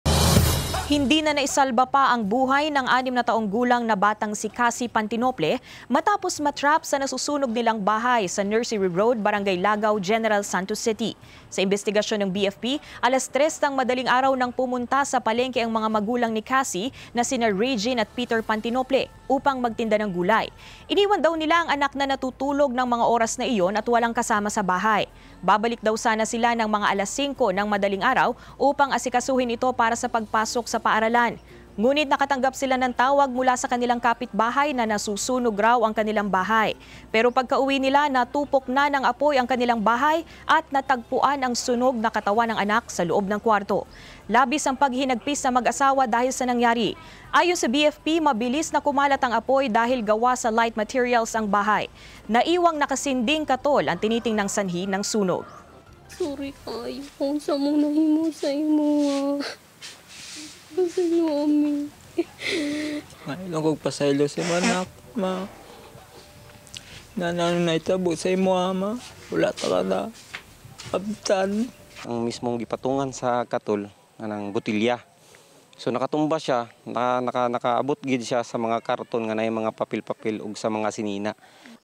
Regional TV News: Trahedya sa Sunog
6-anyos na batang babae, patay matapos ma-trap sa nasusunog na bahay sa General Santos City. Nakasinding katol, tinitingnang sanhi ng sunog ayon sa BFP.